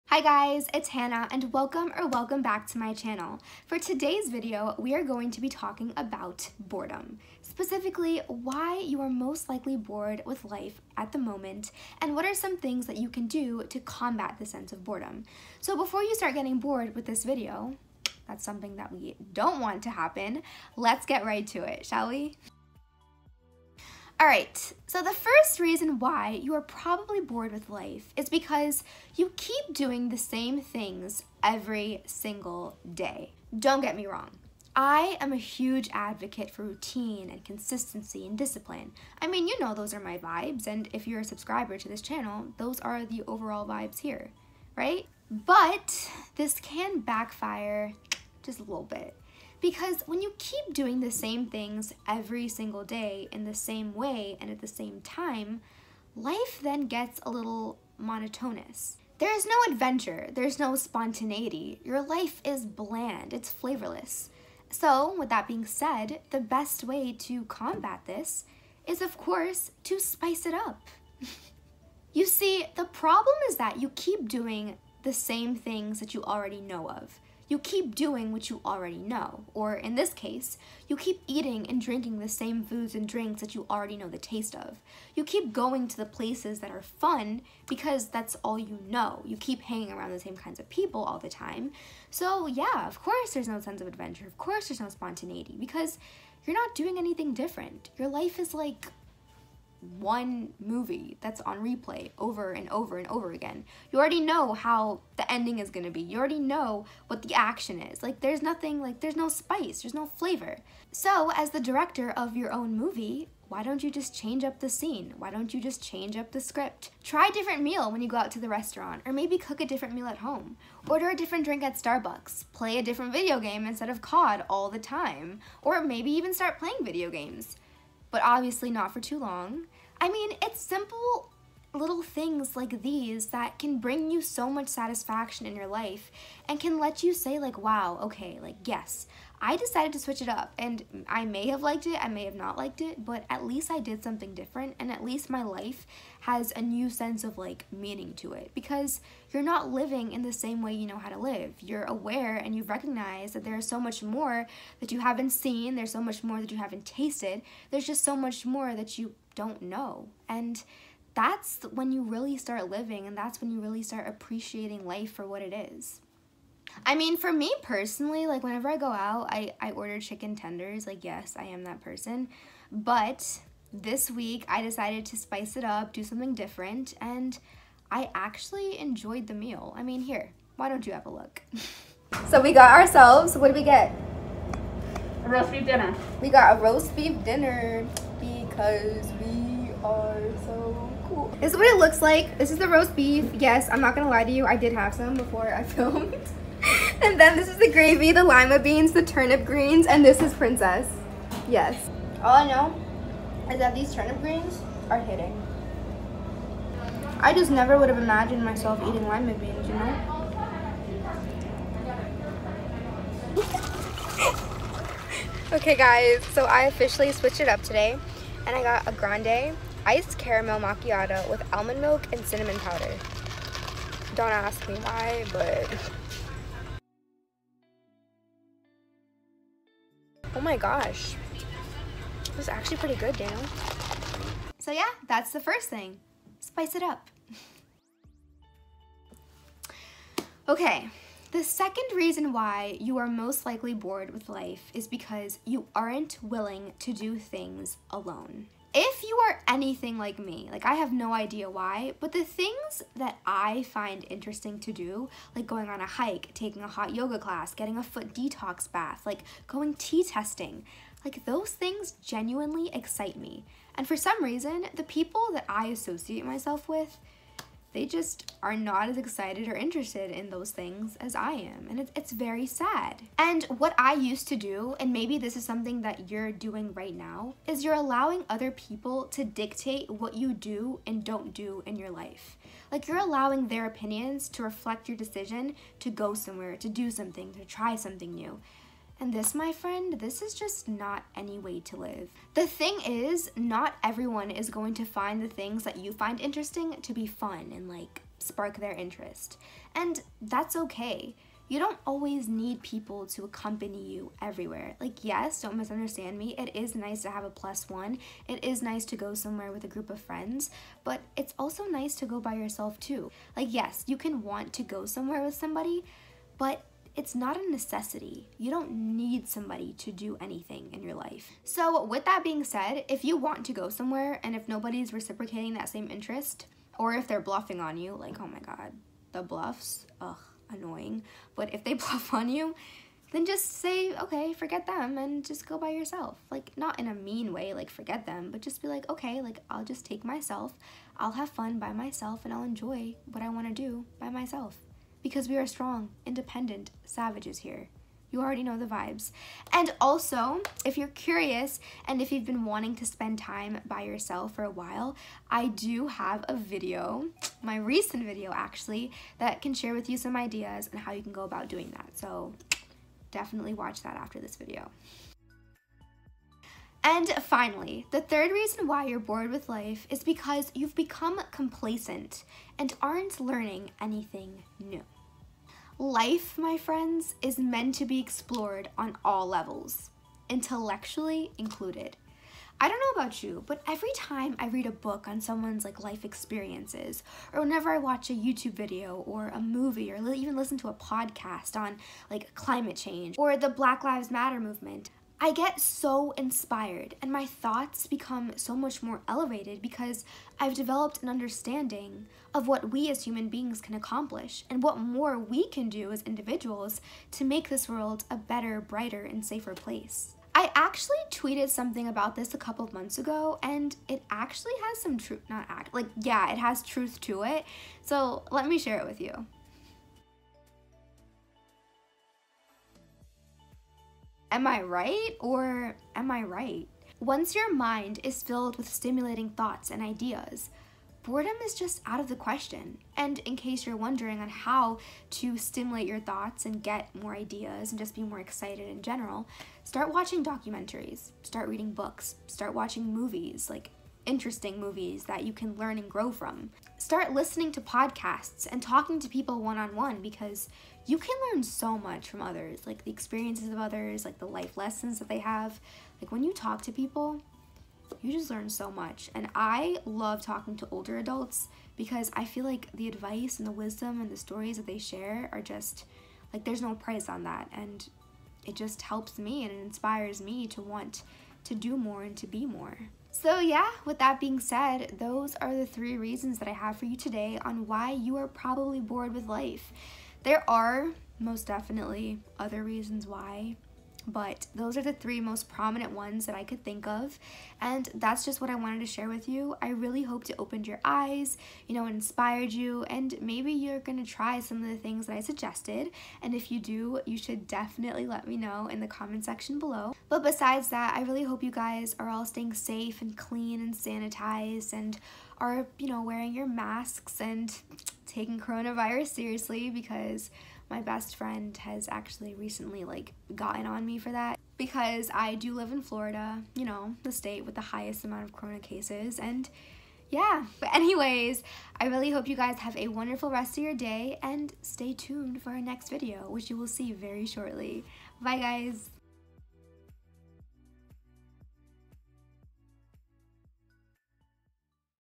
You’re bored with life? Why your life is boring and what you can do to stop feeling bored
In this video you will learn some reasons why you are bored with life and what you can do to stop feeling bored. Here’s a quick summary, but please watch the entire video for more detail!

1.You keep doing the same things every single day; there’s no spontaneity or adventure in your every day routine, so the best way to combat this is to spice up your life.

2.You aren’t willing to do things alone. Not everyone is going to find the things you find fun to be interesting and that’s okay. You can ask people to accompany you somewhere because having a plus 1 makes going out enjoyable, but it’s most definitely not a necessity.

3.You’ve become complacent and aren’t learning anything new. Life my friends is meant to be explored on all levels, intellectually included.

5 ways to spend quality time by yourself | enjoy being alone in your own company and solitude

 Stay clean, stay safe, and stay woke,
Hannah x